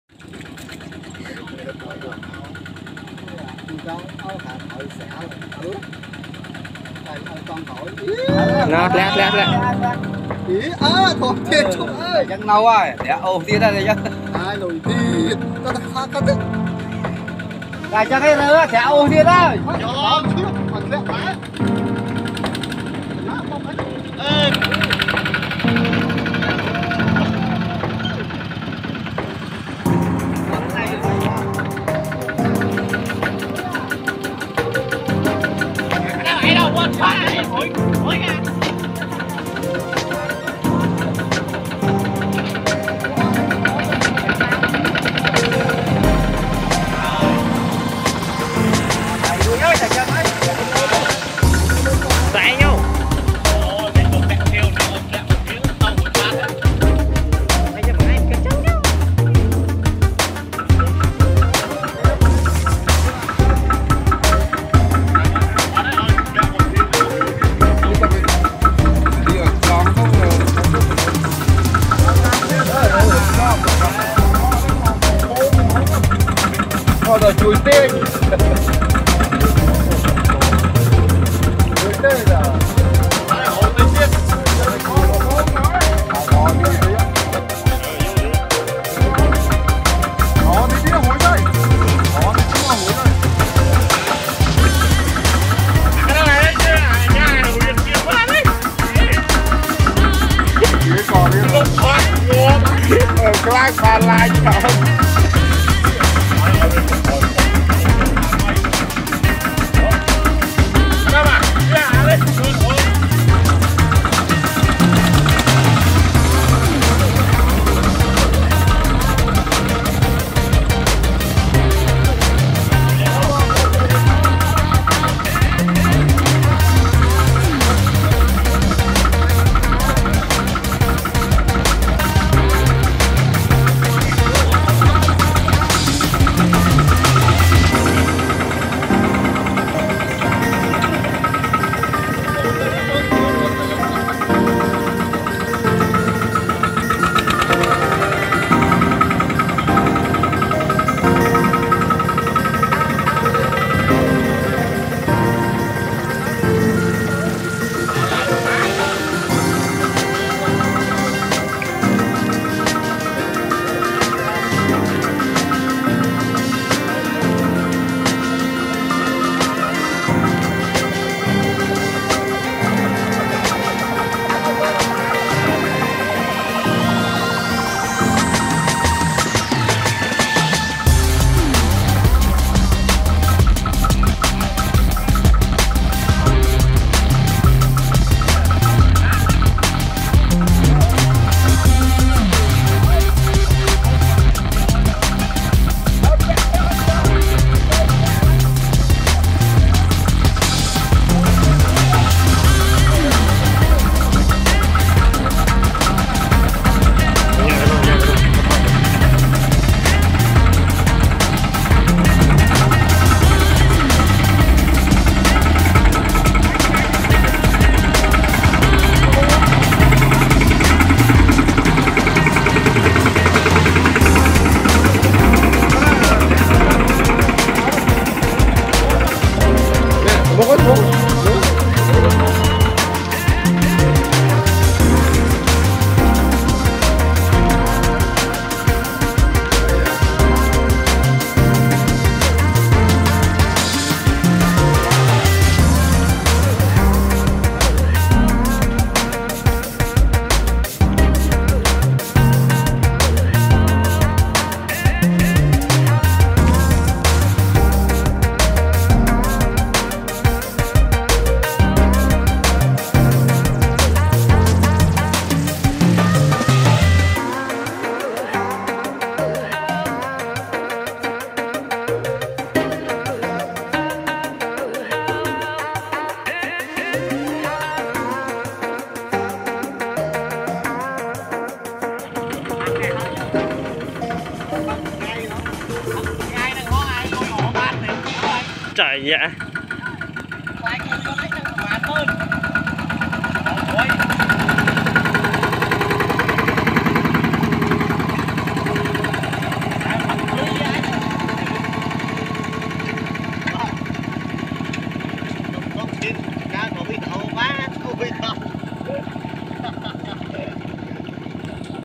Kubota RT120 stuck on the way /
this video will show you about tractor skill and let you know how many power of tractor.